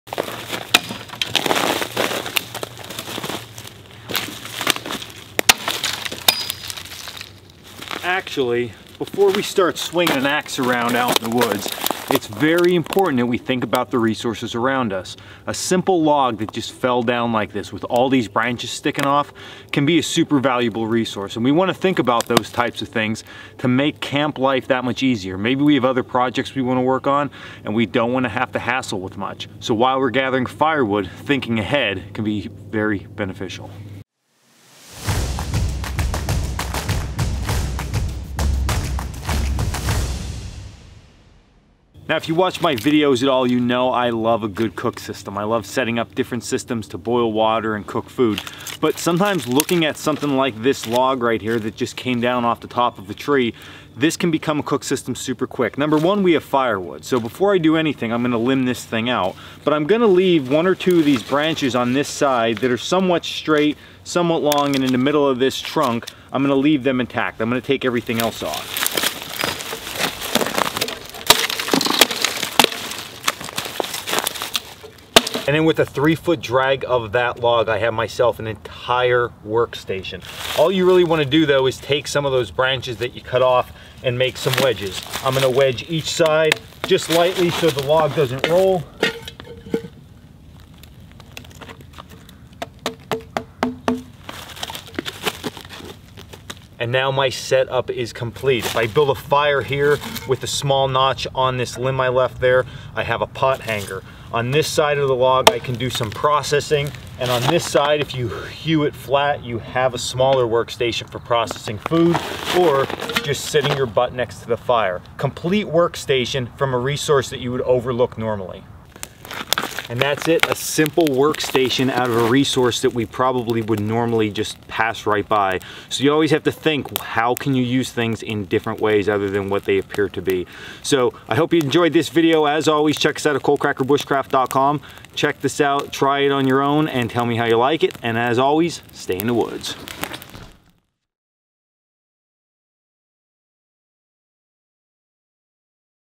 Complete Bushcraft Workstation
Set up a cook system, seat, table, and wood chopping station in a matter of 2 minutes.

Dan